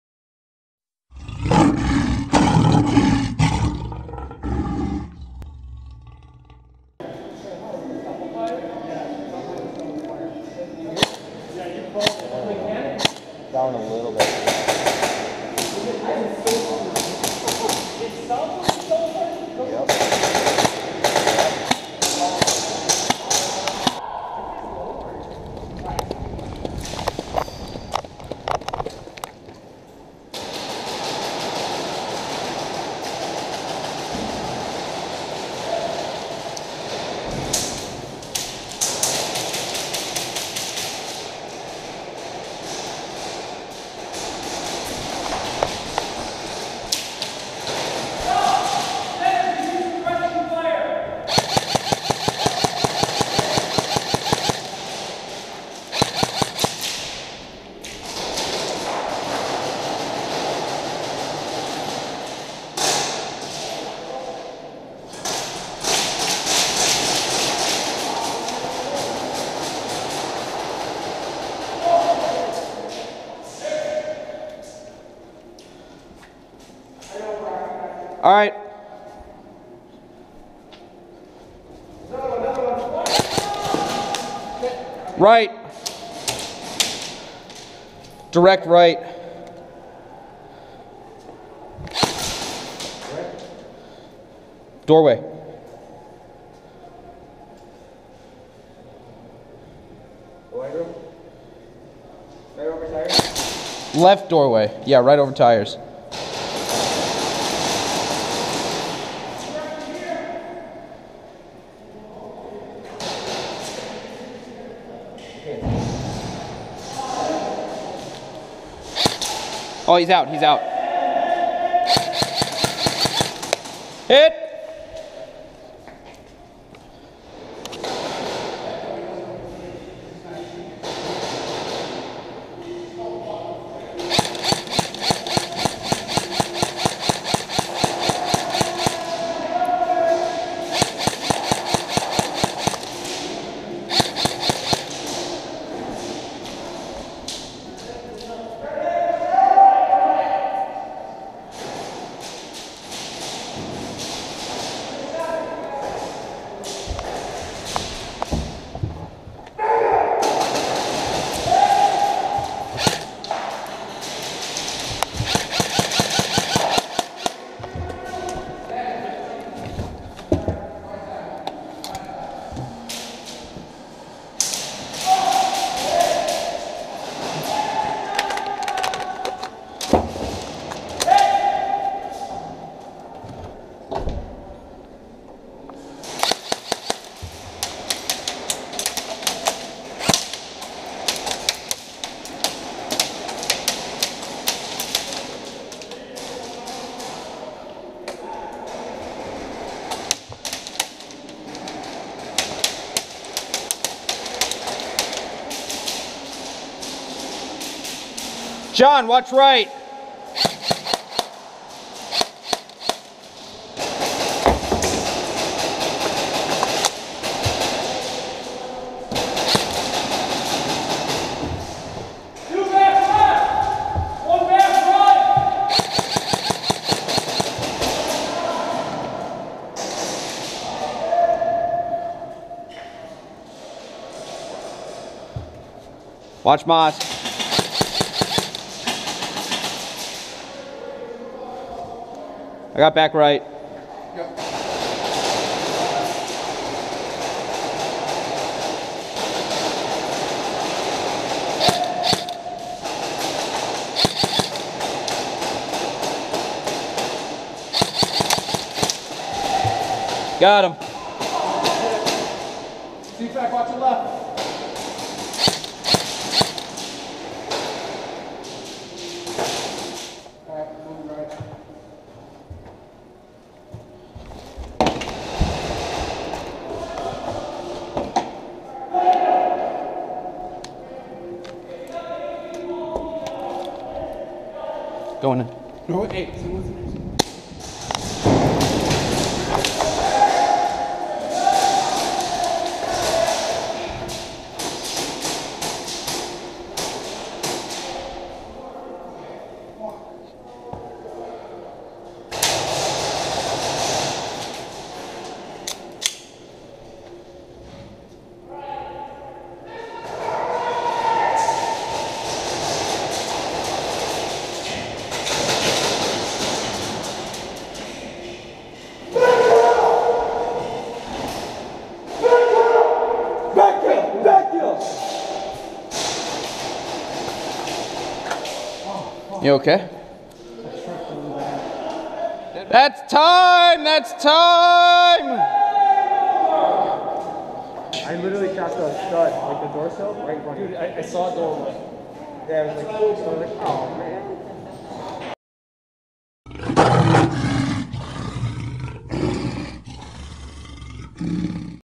The Airsoft Arena in Milwaukee - After Hours Deathmatch: Round 1
SOTRF and friends head to the Airsoft Arena in Milwaukee for some practice in CQB.